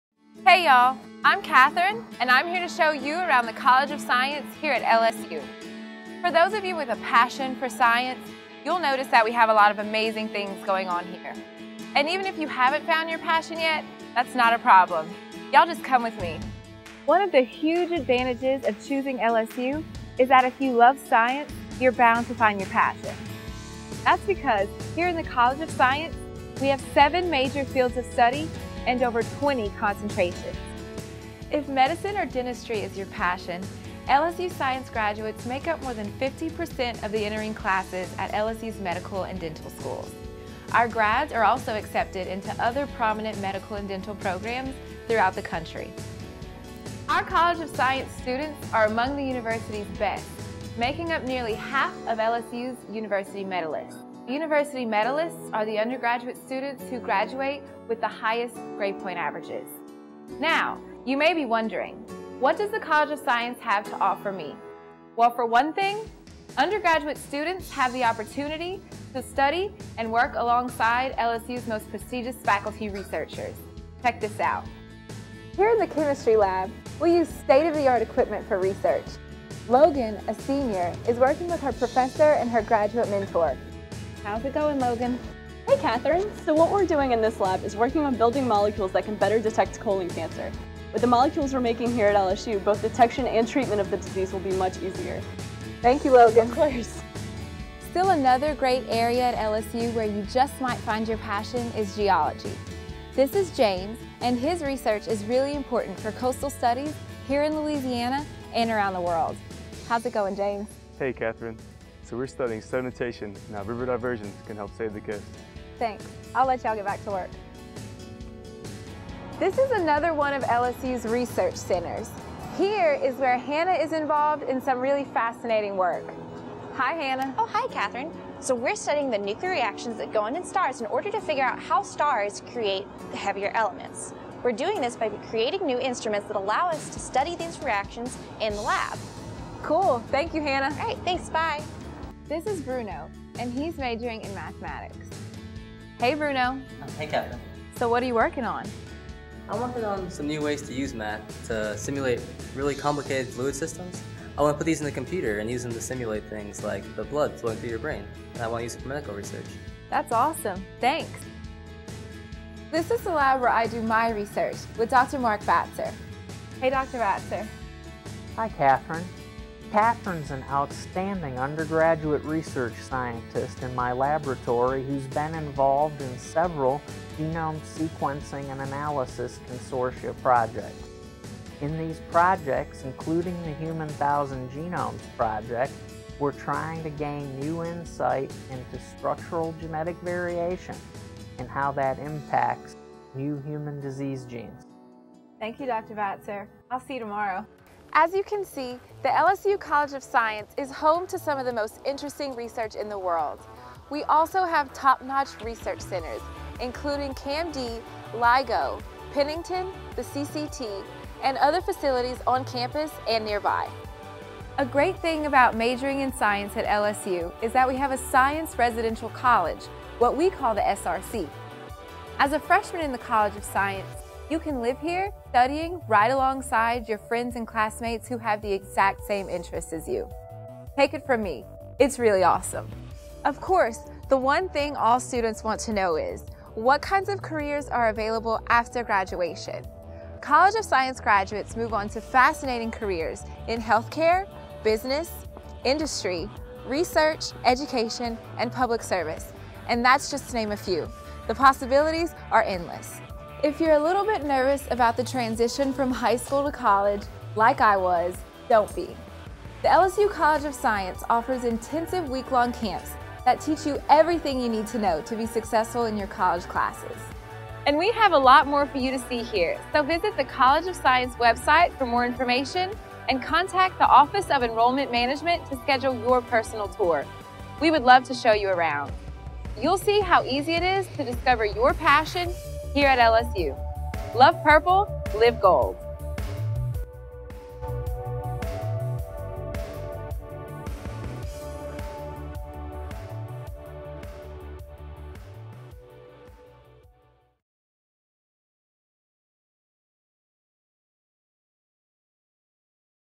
What can the LSU College of Science offer you?
If you are looking for a world-class educational experience and opportunities to work with some the nation's top faculty researchers, then the LSU College of Science is the place for you. The college offers seven major fields of study and more than 20 concentrations.

Whether you are a first-year student or pursuing your graduate studies, the LSU College of Science can help you discover your passion.